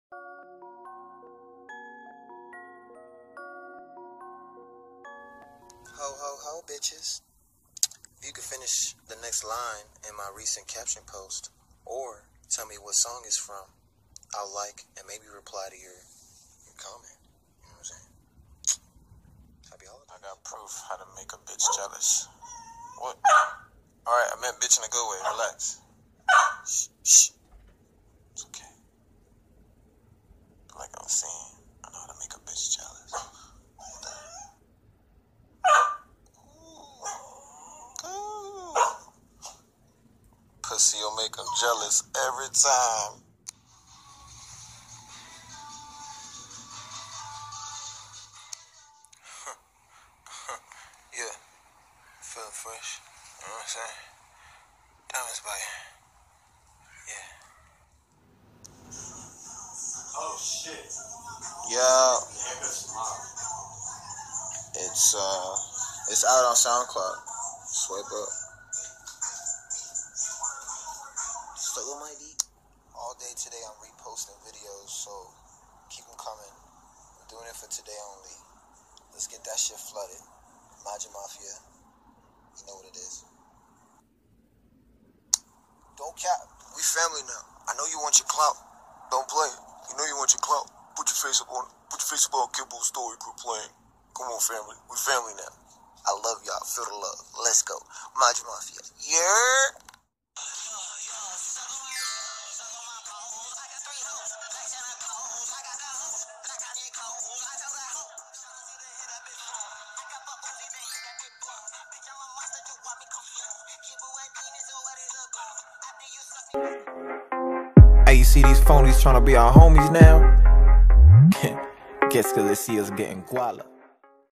Kid Buu Says Happy Holidays & Shows You How to Make a "Bitc#" Jealous!
And more!  I'm still working on the Story of Kid Buu part 3 so be patient with me.  Tyvm for all the love. Makes me want to keep making videos even more.  Happy holidays!